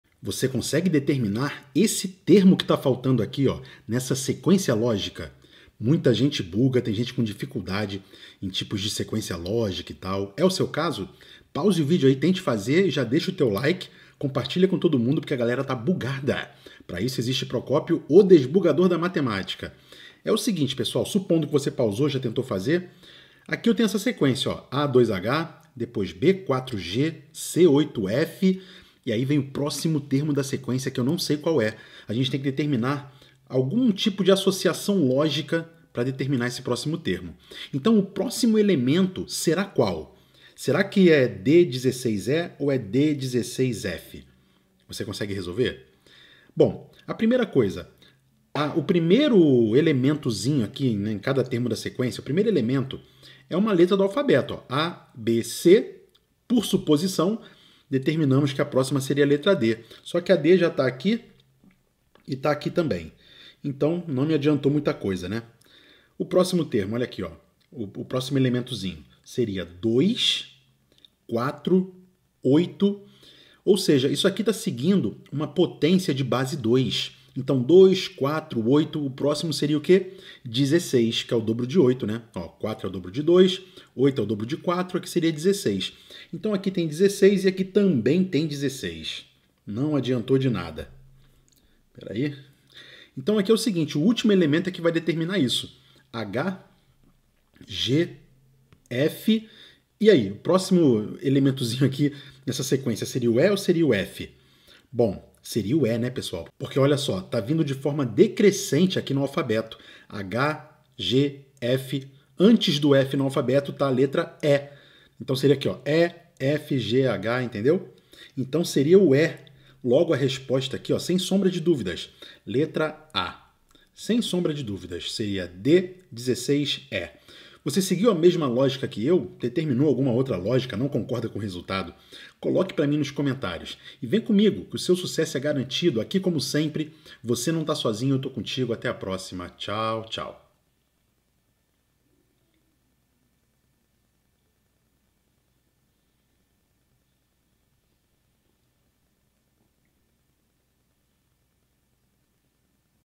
🔥 SEQUÊNCIA LÓGICA: Qual é o PRÓXIMO TERMO? Você tem esse RACIOCÍNIO LÓGICO MATEMÁTICO?
Venha fazer parte dos 100 membros fundadores do novo movimento que vai te levar ao 800+ na prova de Matemática do ENEM. ⭐️

Vem com o Procopio Desbugador da Matemática!